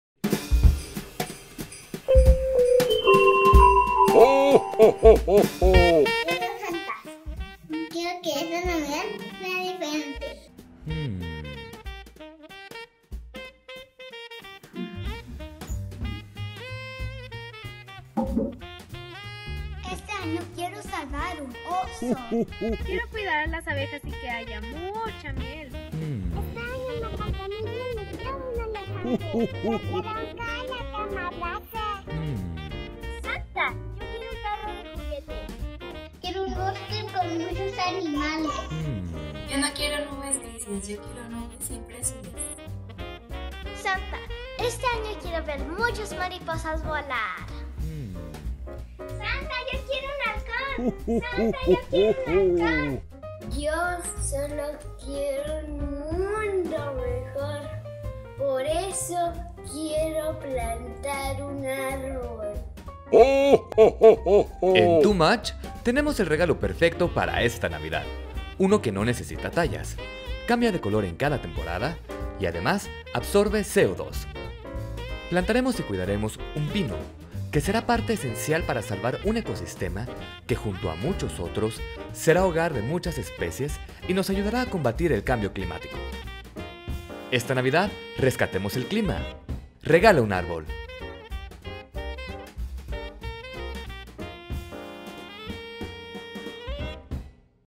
EL REGALO PERFECTO
Esta navidad el regalo perfecto es un árbol, absorbe CO2, combate el cambio climático y lo puedes hacer desde la palma de tu mano.

Visita nuestro sitio elige un proyecto y salvemos al clima!

Se un climactor de 2ºmuch!